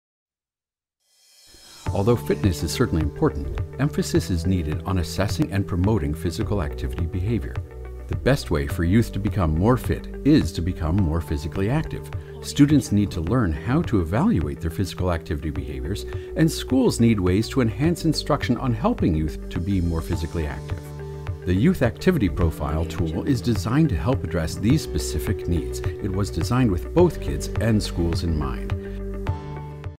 YAP Introduction
A brief introduction to the Youth Activity Profile tool.